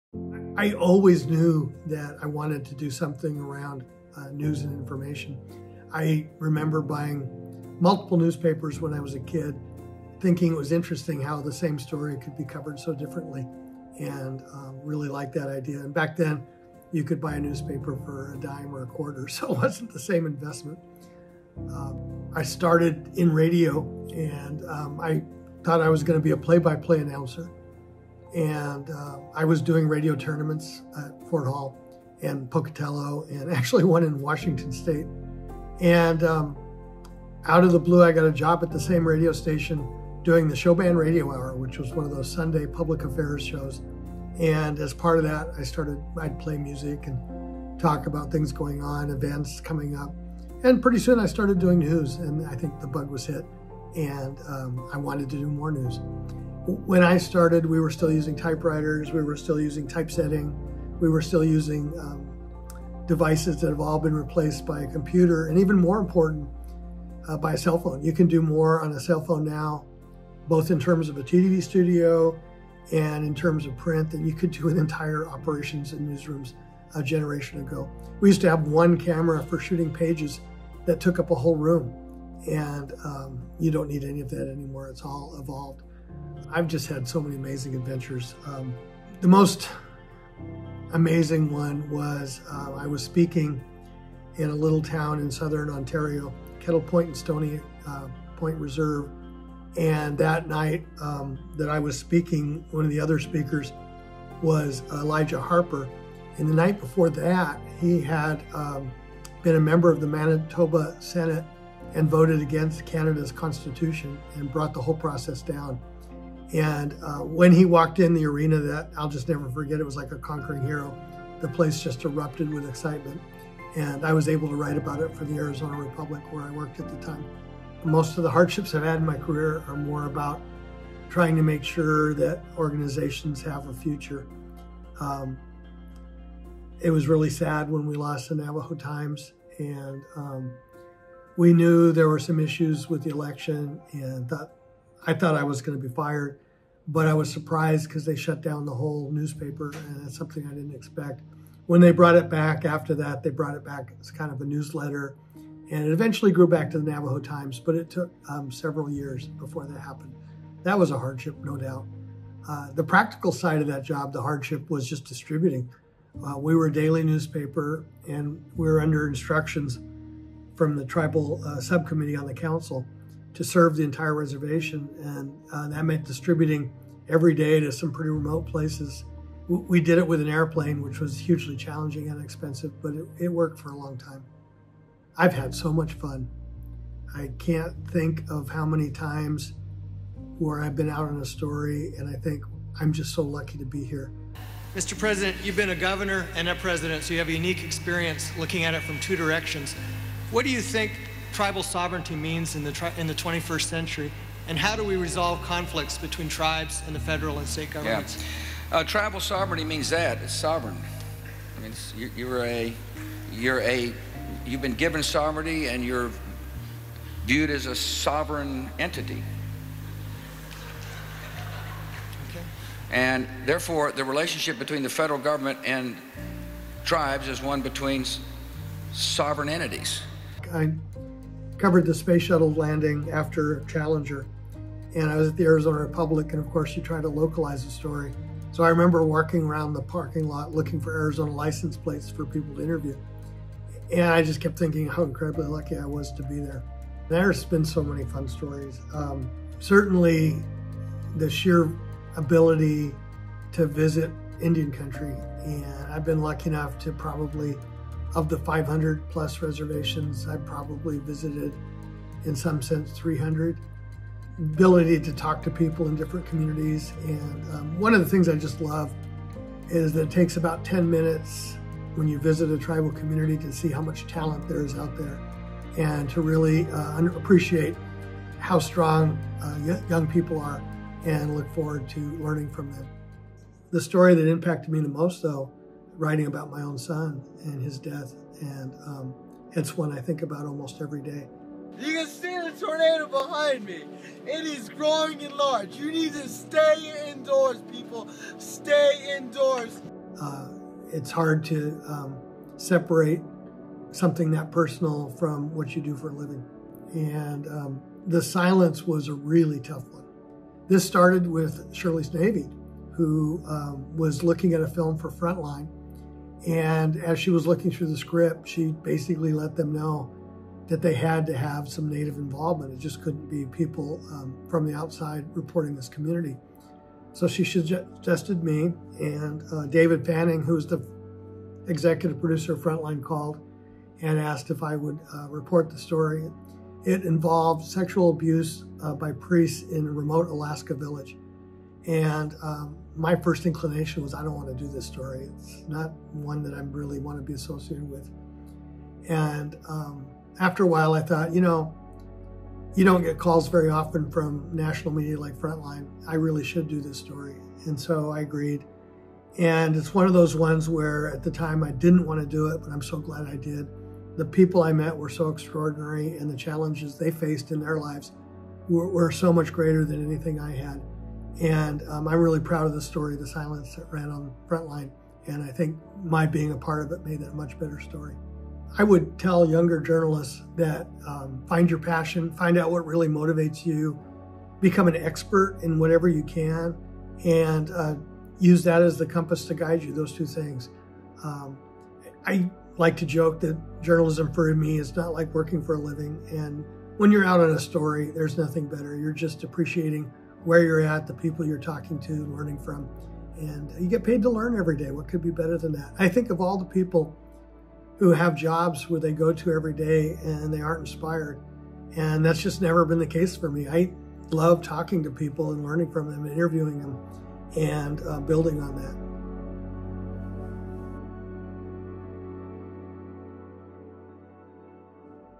50 Years in News with Mark Trahant
From covering a historic space shuttle launch to the ripple effects an election had on the Navajo Times, ICT's Mark Trahant’s journalism career has been a front-row seat to history’s most pivotal moments.

Don't miss Mark (@trahantreports) as he recounts the uplifting, challenging and once-in-a-lifetime stories he’s covered in the last 50 years.

Produced and edited by Quindrea Yazzie, ICT